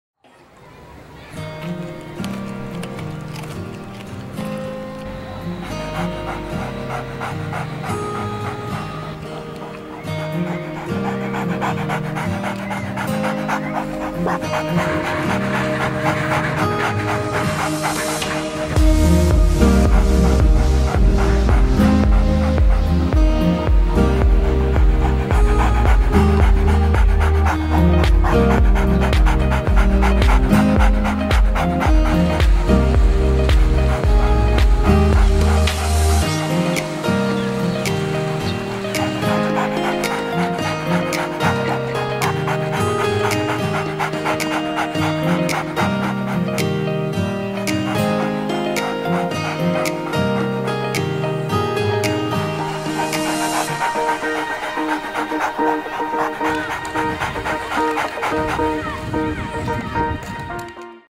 My Best Friends (Canon XHA1 in 2021)
A little tribute to the dogs in my life. They are all very special to me. I shot this on my Canon XHA1 camcorder in 2021.